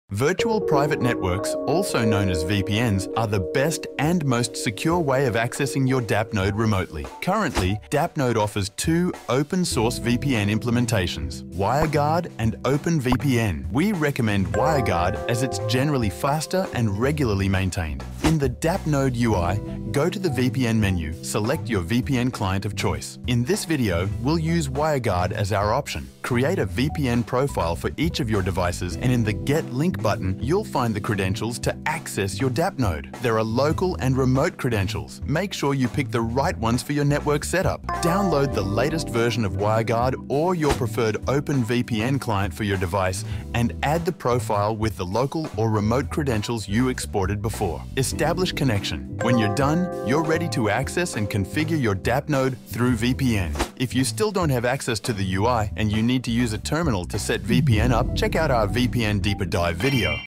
Dappnode Driving School - VPN Configuration
In this video tutorial, we delve into the world of Virtual Private Networks (VPNs) and how they offer the most secure way to access your Dappnode remotely.

This tutorial covers:
1. VPN Options: Understanding Wireguard and OpenVPN.
2. VPN Profile Setup: Creating and downloading profiles for each device.
3. Connection Process: Step-by-step guide to connect using Wireguard or OpenVPN.
4. Accessing Dappnode: Navigating the Dappnode UI through VPN.

Designed for both beginners and advanced users, this guide makes setting up VPN on your Dappnode fast and easy. Follow along as we walk you through every step, ensuring a secure and seamless connection to your Dappnode from anywhere.

Don't forget to check out our VPN Deeper Dive video for more detailed instructions on terminal access!